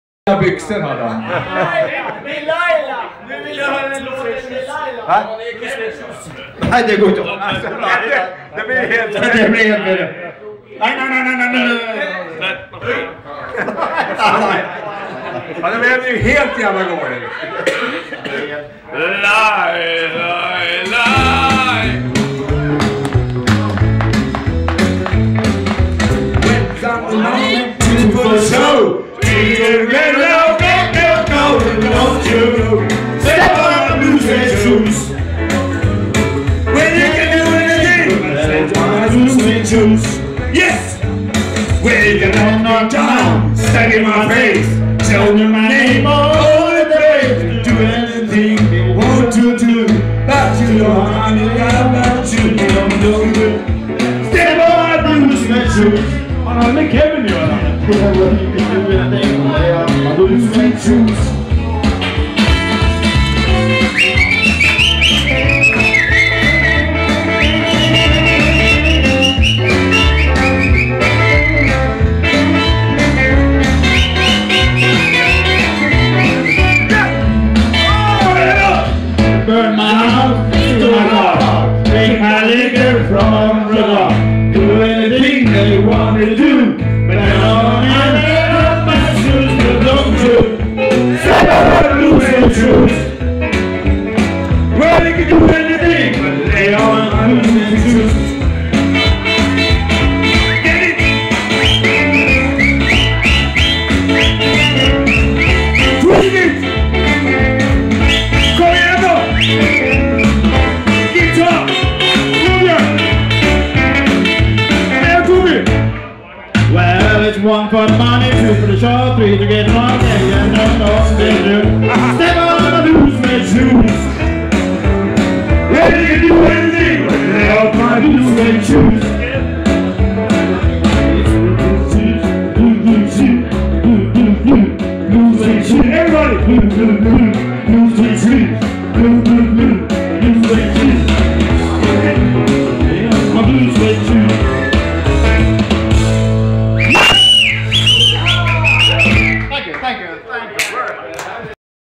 Tore og Bengt "synger" karaoke
Karaoke på Banken Bar Stjørdal, Union Cup Bankett 2008